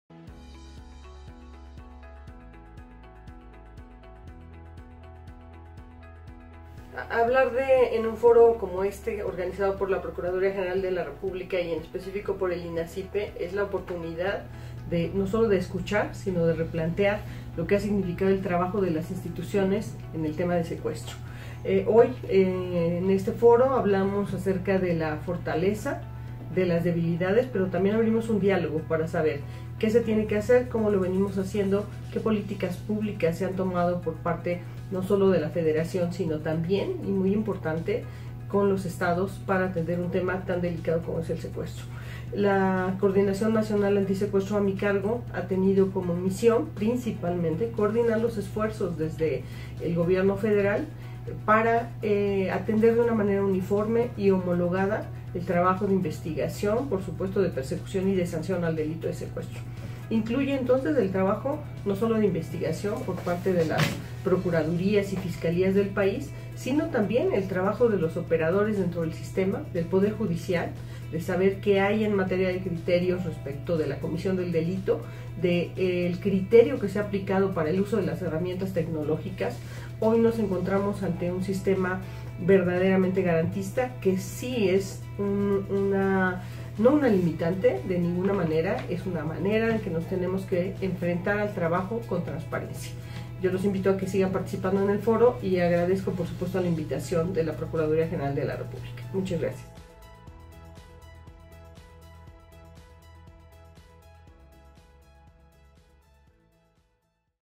Instituciones, políticas públicas y eficiencia gubernamental en el combate al secuestro
Patricia Bugarín Instituciones, políticas públicas y eficiencia gubernamental en el combate al secuestro